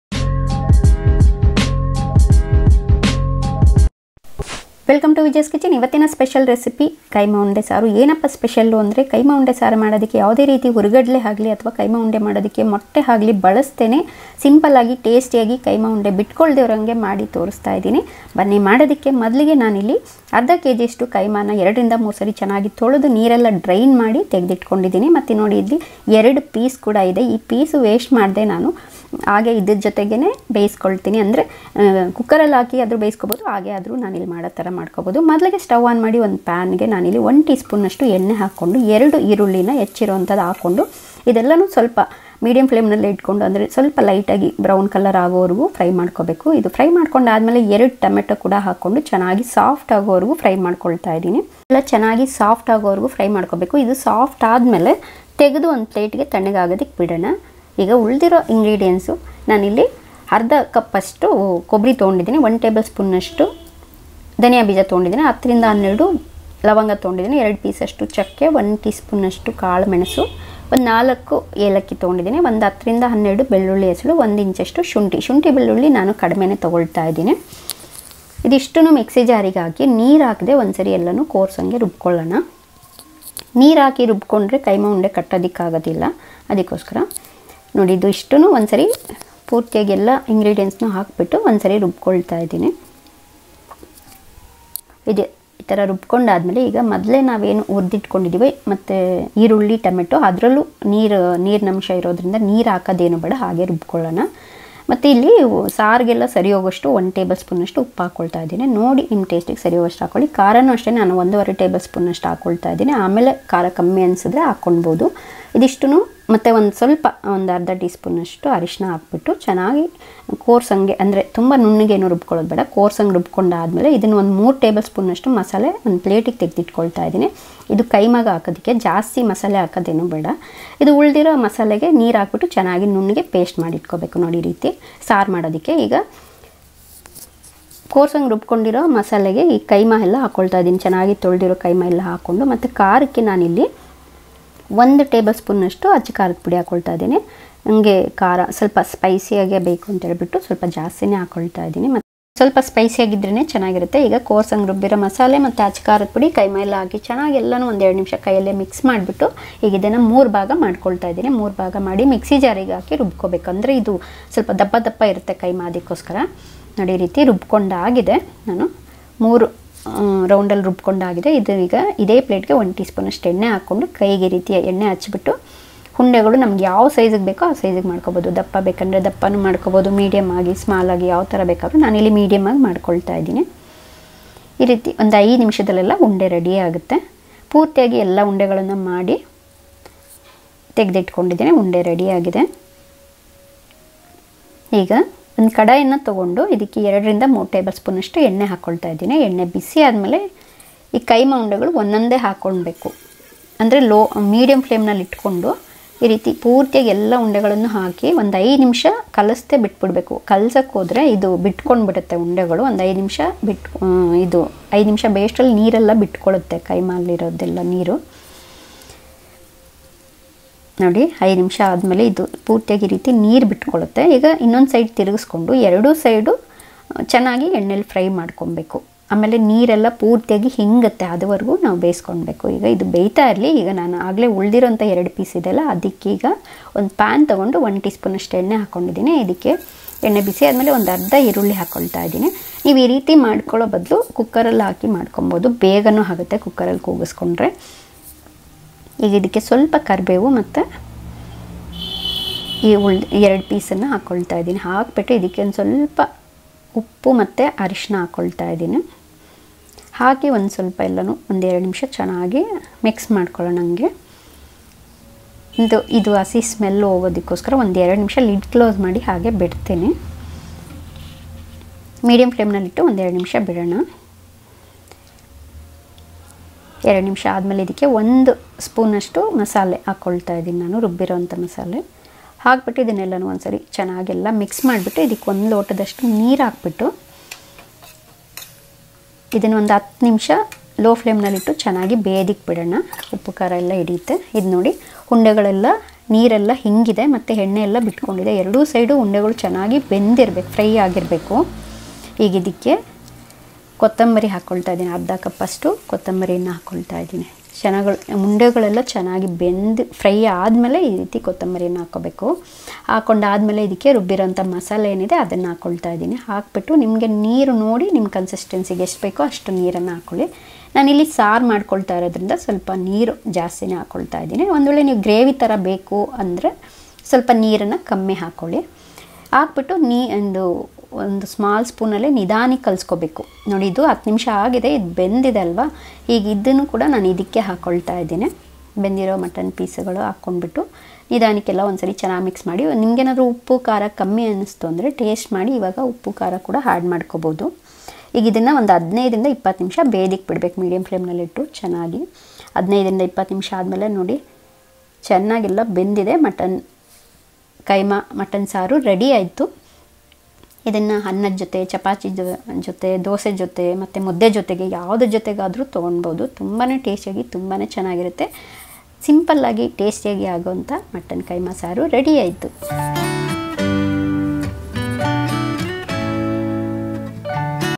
ರುಚಿಯಾದ ಮಟನ್ ಕೈಮಾ ಉಂಡೆ ಸಾರು/ Tasty Mutton Kheema balls curry |keema curry recipe
Kaima unde saaru or mutton keema balls curry is a traditional Karanataka style recipe, it is usually known as meat balls curry.
Goes well with ragi muddhe,white rice,pulav,ghee rice and also with chapathi,akkirotti dosa,idli,poori….

       INGREDIENTS:-
Mutton Kheema -½ kg
Fresh coconut    - ½ cup
Coriander seeds-1tbsp
Cloves. -10 to 12
Cinnamon-pieces ( 1inch)
Cardamom-4
Ginger-1 inch
Garlic-10to 12
Pepper corn-1tsp
Turmeric powder -1tsp
Red chilli powder-3 to 4 tbsp
Onion-2 no’

Thankin you :)
Vijaya’s kitchen